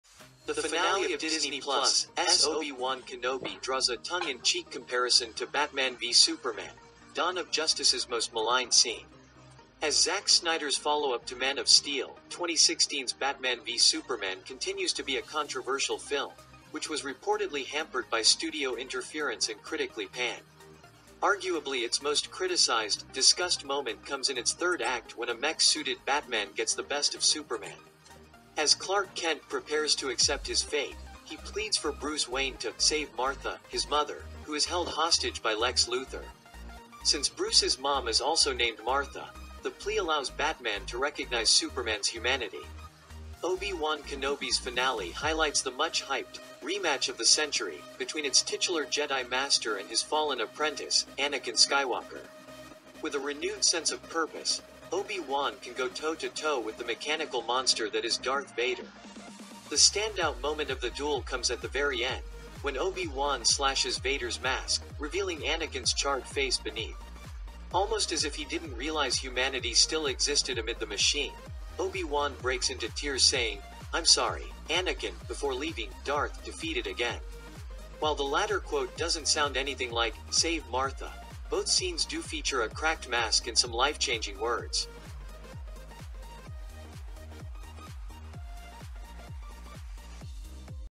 Obi Wan Kenobi Finale Draws Comparison to BvS Most Hated Scene
The finale of Disney+'s Obi-Wan Kenobi draws a tongue-in-cheek comparison to Batman v Superman: Dawn of Justice's most-maligned scene. As Zack Snyder's follow-up to Man of Steel, 2016's Batman v Superman continues to be a controversial film—which was reportedly hampered by studio interference and critically panned. Arguably its most criticized/discussed moment comes in its third act when a mech-suited Batman gets the best of Superman. As Clark Kent prepares to accept his fate, he pleads for Bruce Wayne to "save Martha" – his mother, who is held hostage by Lex Luther. Since Bruce's mom is also named Martha, the plea allows Batman to recognize Superman's humanity.